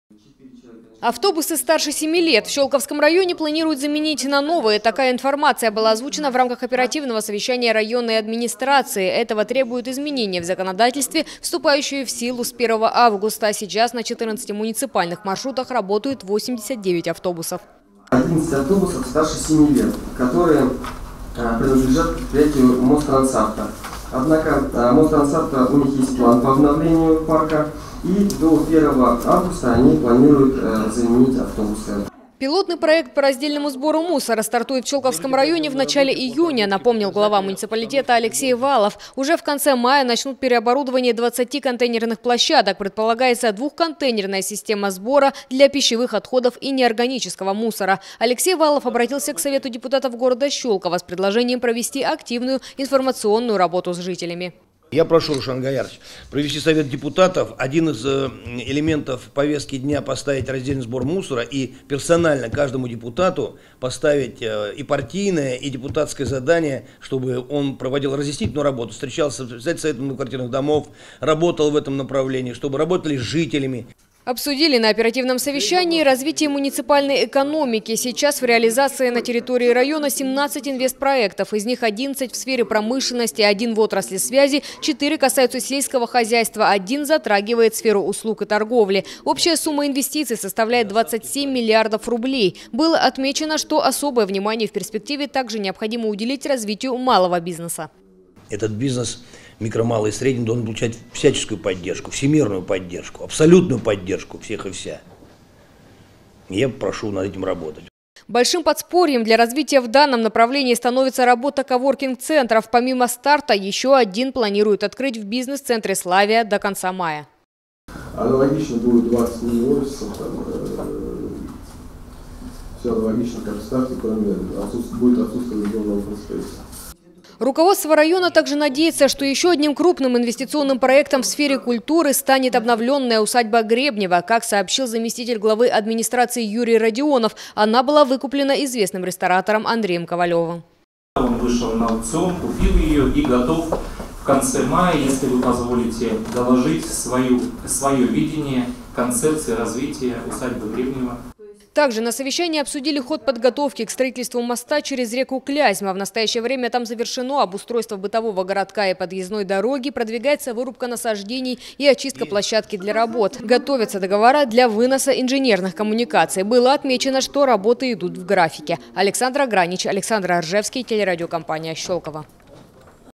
В Щёлковском районе заменят автобусы старше 7 лет
Автопарк общественного транспорта в Щёлковском районе ждёт обновление, об этом рассказали на оперативном совещании администрации Щёлковского района. Также на заседании обсудили ход реализации инвестиционных проектов и подготовку к строительству моста через Клязьму.